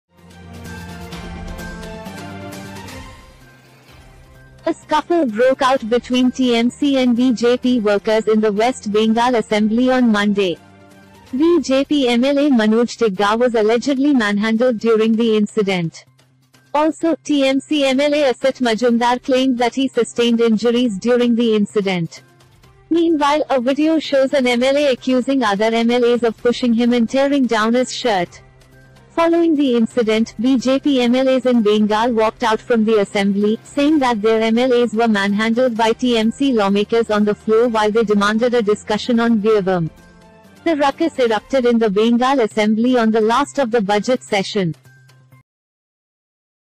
Scuffle between TMC, BJP MLAs inside Assembly over Birbhum incident, 5 suspended
Will Smith punching Chris Rock reminds Twitter of Ashutosh Gowariker blasting Sajid Khan

0:10 Scuffle between TMC, BJP MLAs inside Assembly over Birbhum incident, 5 suspended